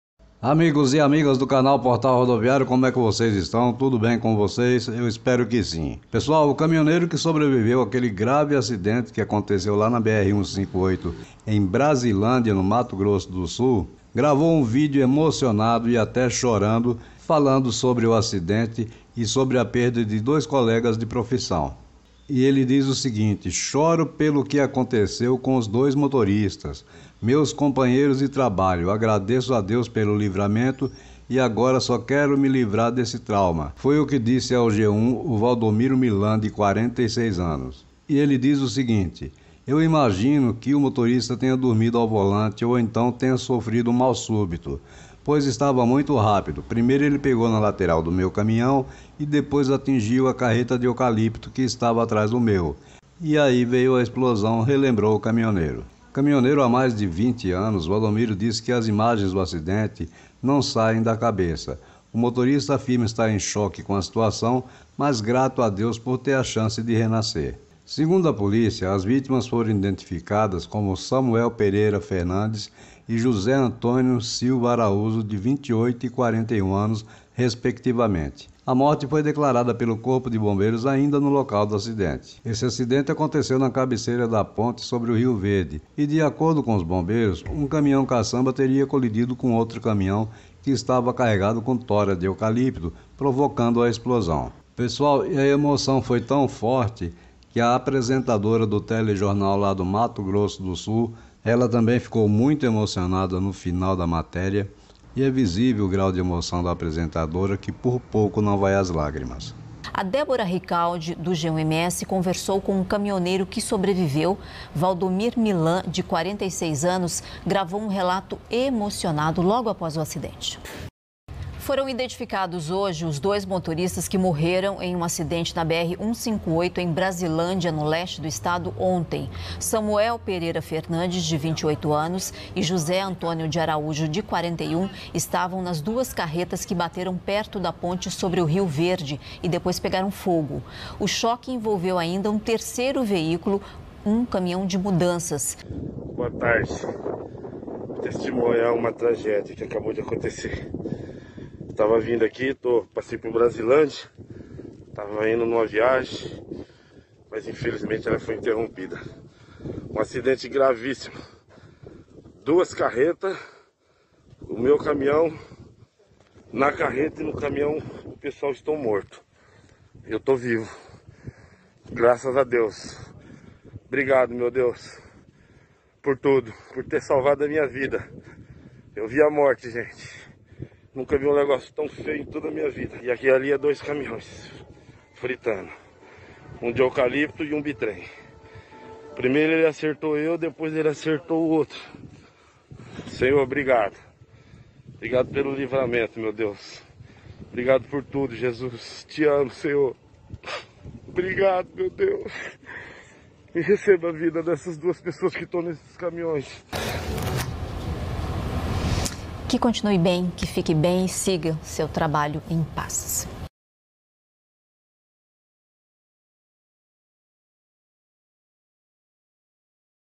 Caminhoneiro chora ao falar do acidente que tirou a vida de dois colegas de profissão
Segundo a polícia, as vítimas foram identificadas como Samuel Pereira Fernandes e Jose Antônio Silva de Araújo, de 28 e 41 anos. A morte foi declarada pelo Corpo de Bombeiros, ainda no local do acidente.

Entenda a dinâmica

Três caminhões explodiram após uma colisão na BR-158, em Brasilândia, a 348 quilômetros de Campo Grande, na tarde desta quinta-feira (5). O Corpo de Bombeiros e a Polícia Militar do município foram acionados e, segundo a Polícia Rodoviária Federal (PRF), duas pessoas morreram no local.